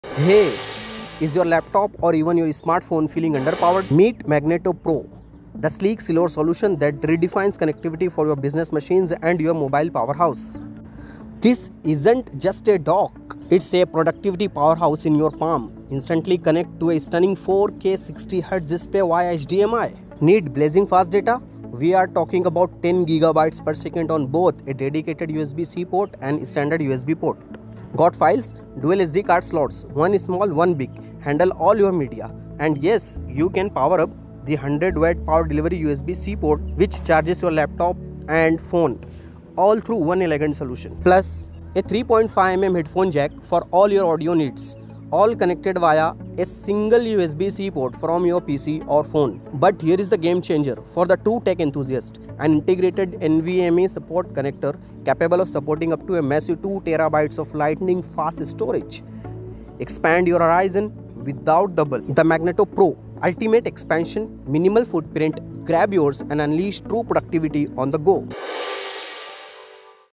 This Magneto Pro Feature Changes EVERYTHING!
The product (Magneto Pro) is a multifunctional magnetic TYPE-C docking station. It has SD/SF card read slots, a PD (100W) power fast charging port; 2*USB3.2 GEN2 (10G) high-speed ports that comply with the USB3.2 standard and support backward compatibility with USB2.0; 1*4K@60HZ display HDMI interface. In addition, the docking station with a built-in M.2 SSD slot that supports NVME protocol 2230 SSD disk reading & writing. The maximum transmission rate can reach USB3.2GEN2 (10Gbps).